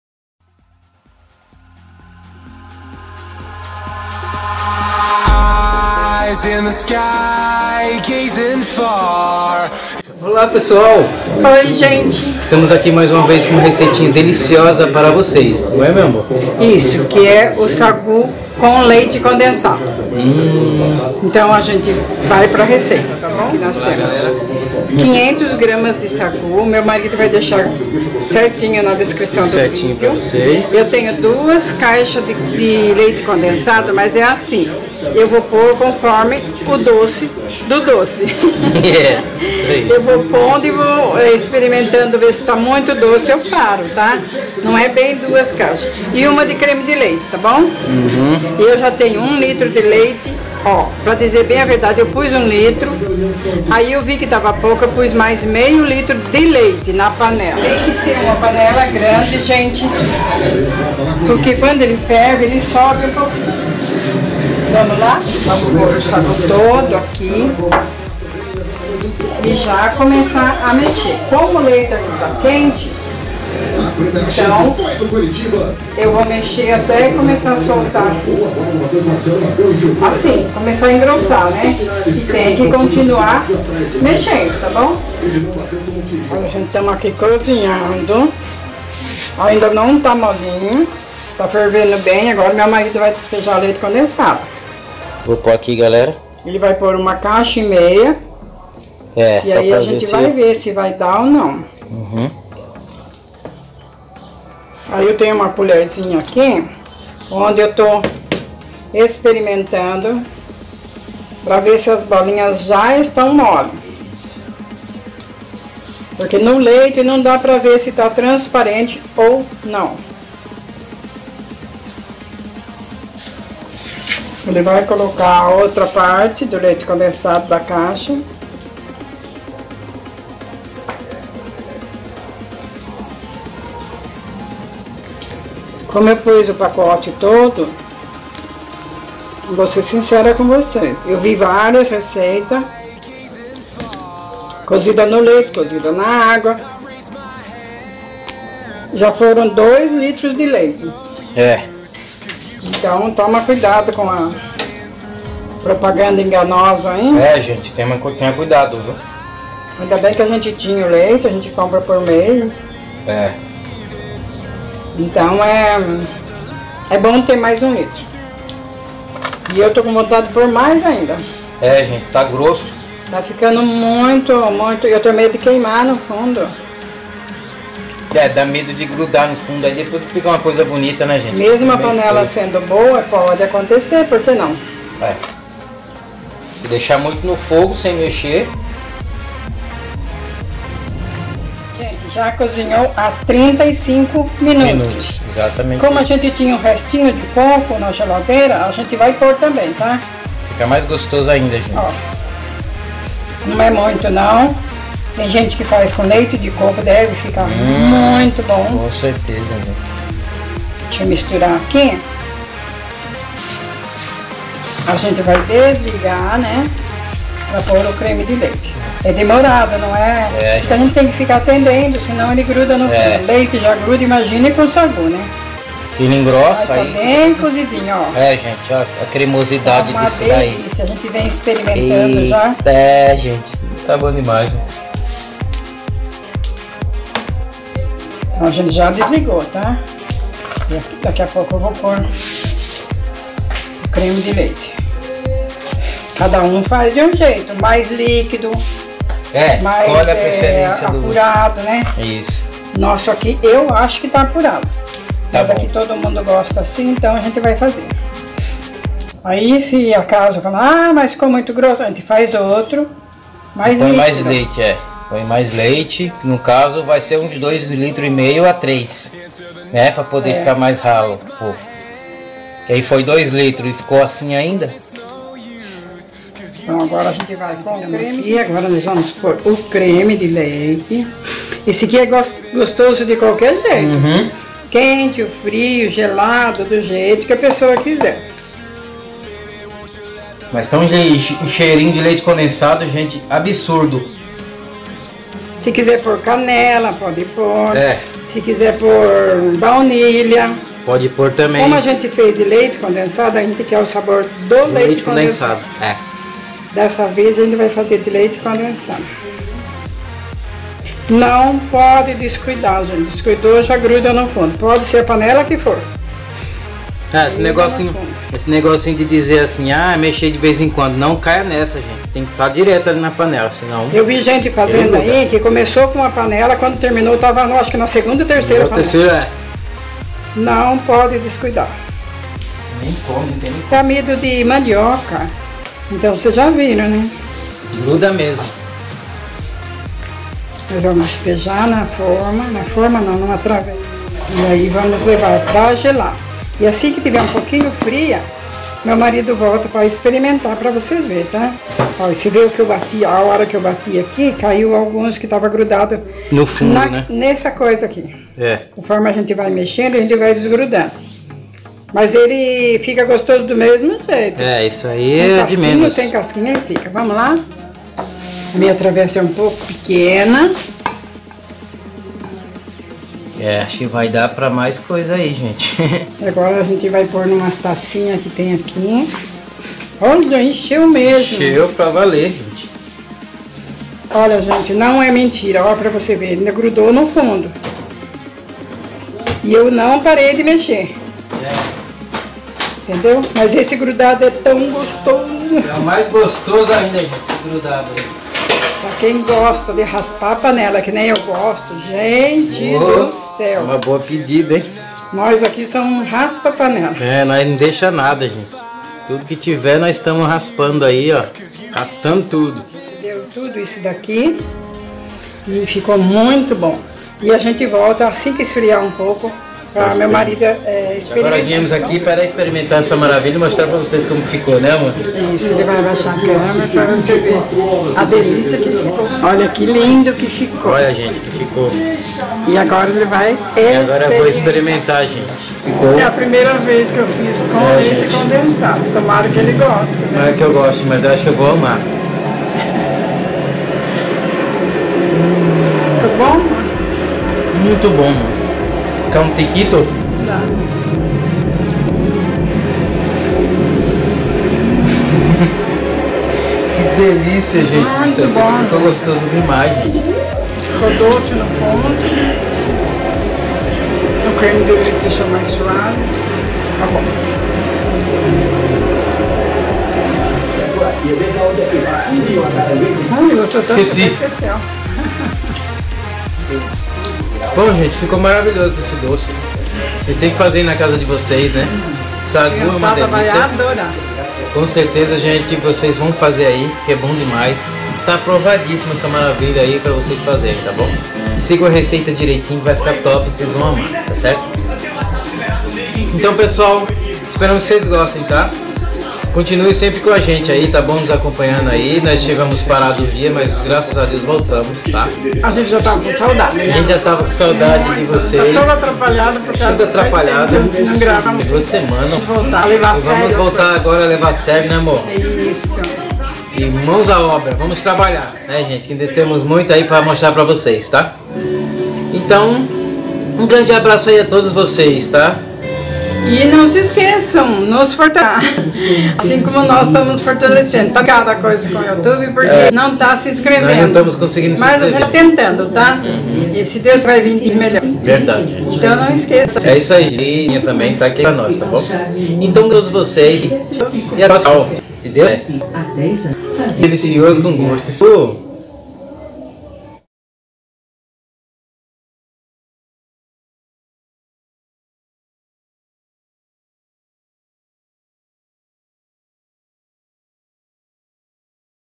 Receita:SAGU COM LEITE CONDENSADO
Ingredientes:
500 gr de sagu
2 caixinha de leite condensado
1 caixinha de creme de leite
2 à 3 litros de leite
Esperamos que gostem..abraços..
CAIXA POSTAL 13
RUA DUQUE DE CAXIAS, 181-CENTRO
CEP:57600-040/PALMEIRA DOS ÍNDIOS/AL